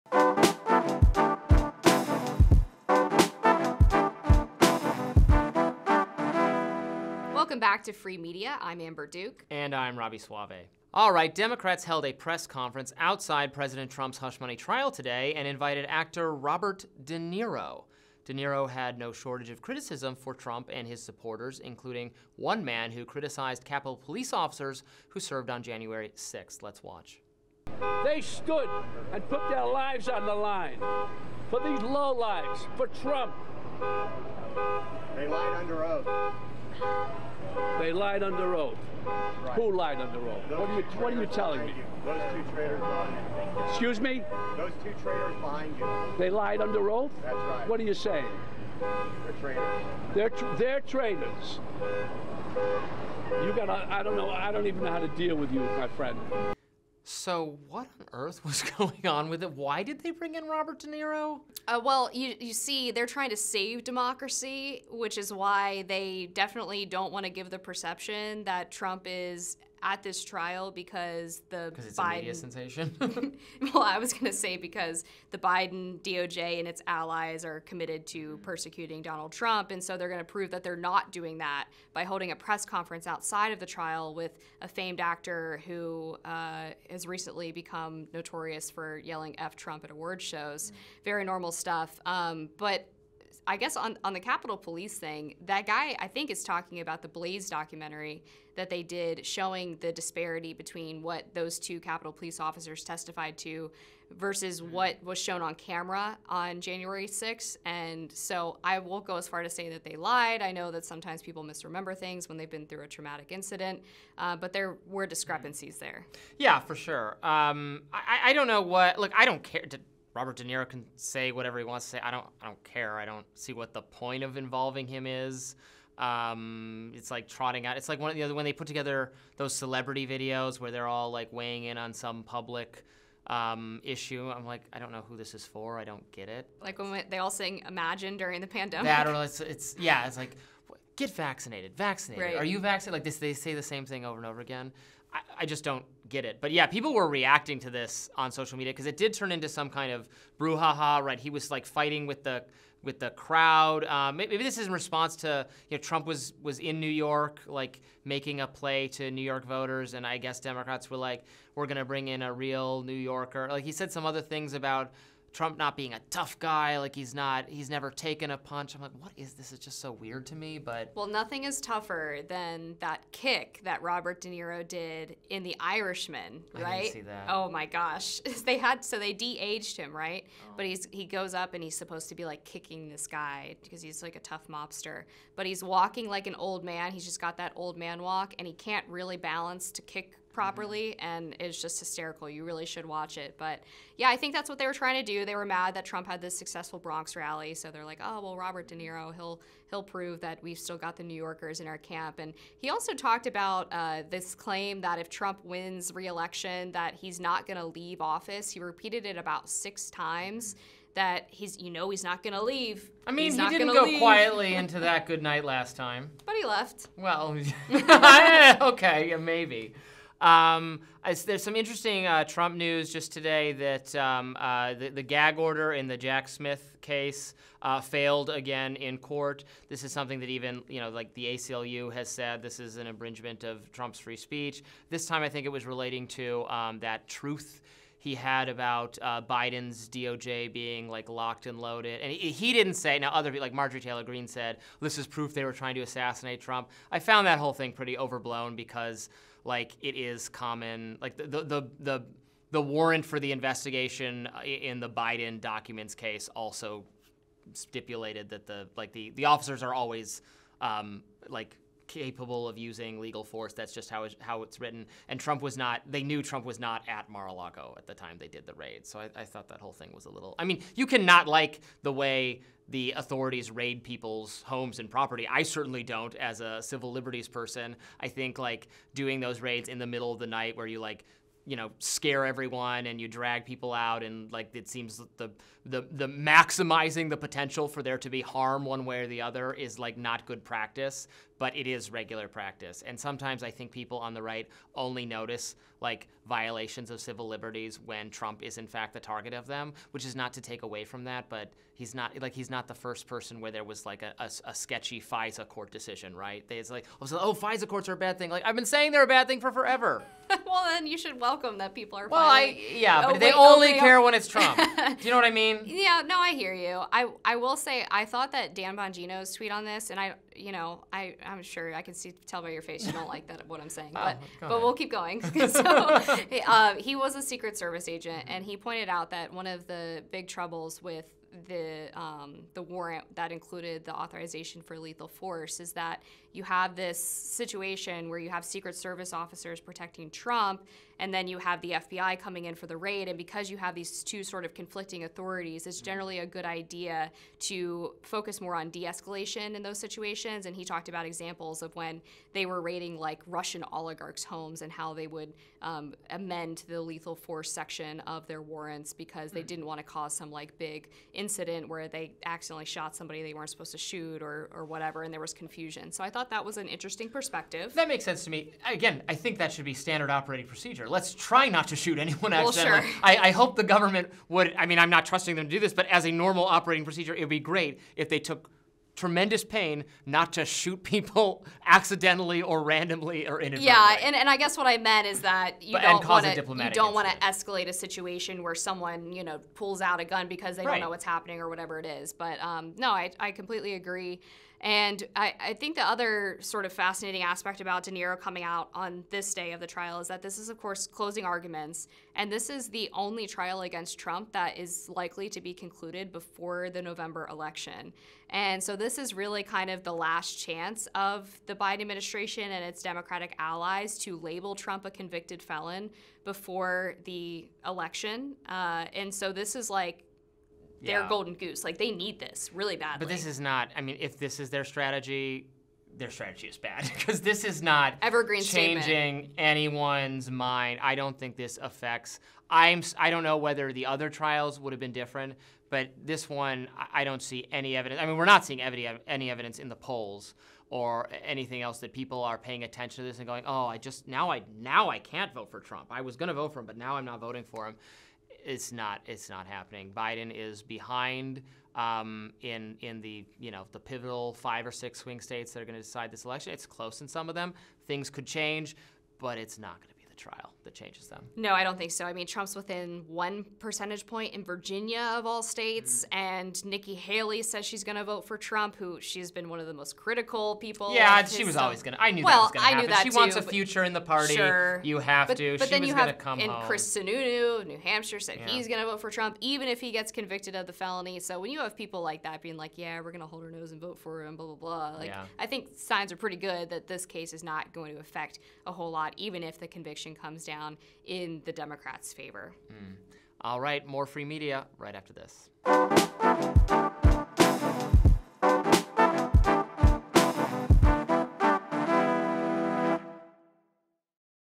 WATCH: Robert De Niro CONFRONTED by Trump supporter outside hush money trial
Robby Soave and Amber Duke discuss actor Robert De Niro's press conference outside the hush money trial of former President Donald Trump.

Director of Photography: Alex Rosen
Producer: Veronica Riccobene
Editor: Regan McDaniel

Illustration: Fox News Network